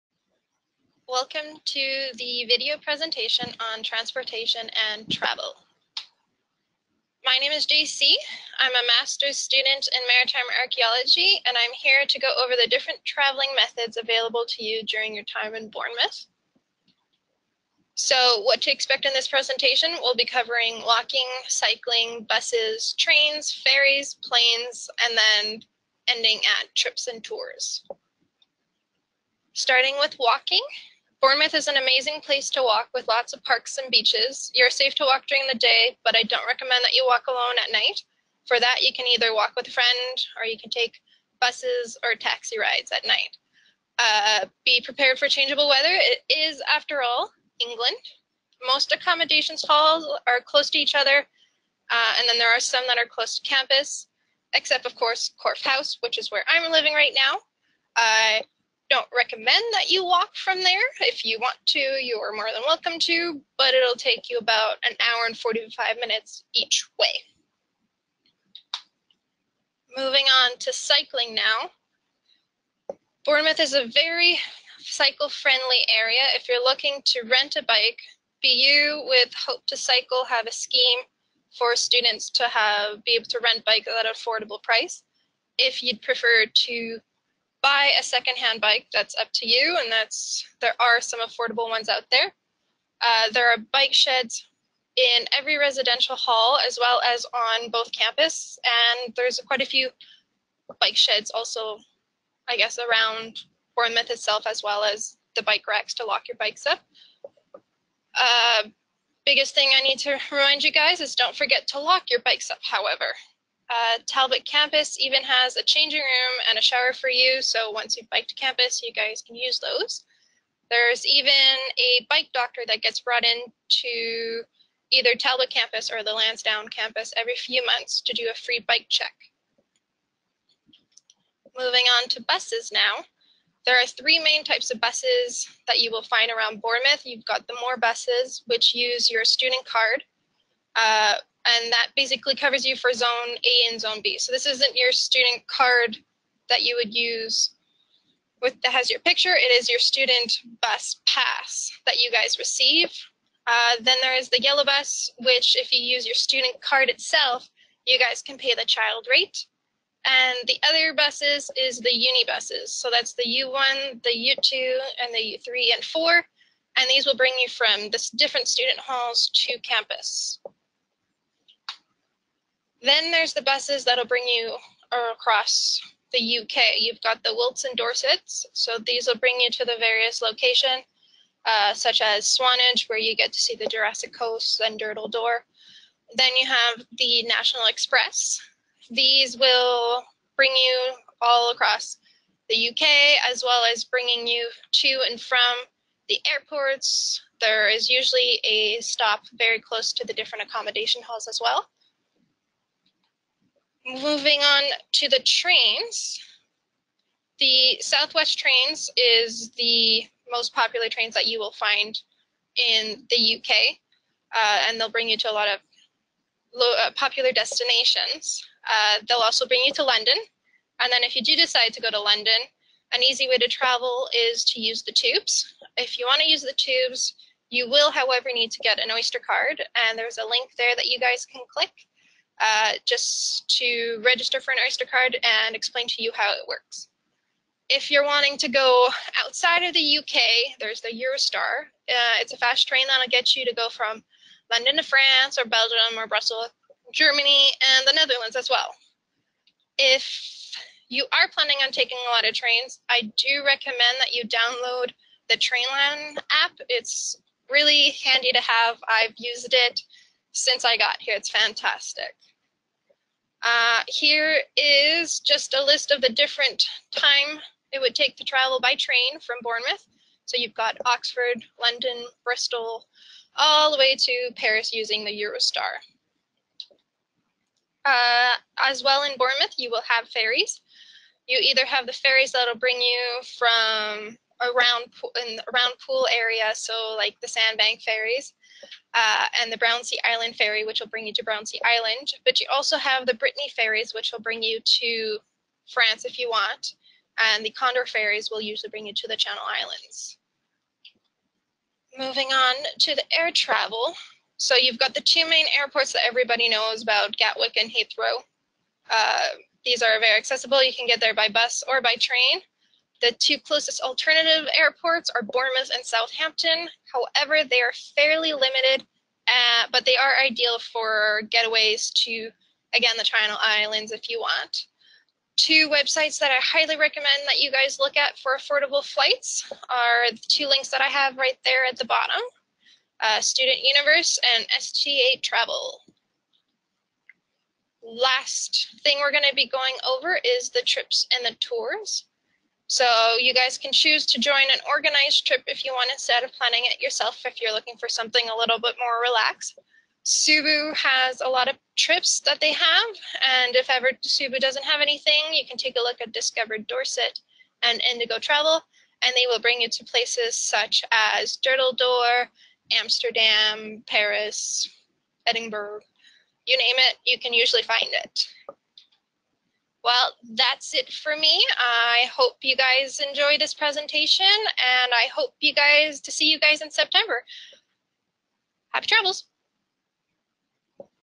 Travel and transportation
Listen to JC Bottomley, a postgraduate student; give a presentation on travelling in Bournemouth and the UK. There are multiple ways to get around the UK, which can seem daunting, but here are some helpful tips and links to relevant information to help you out.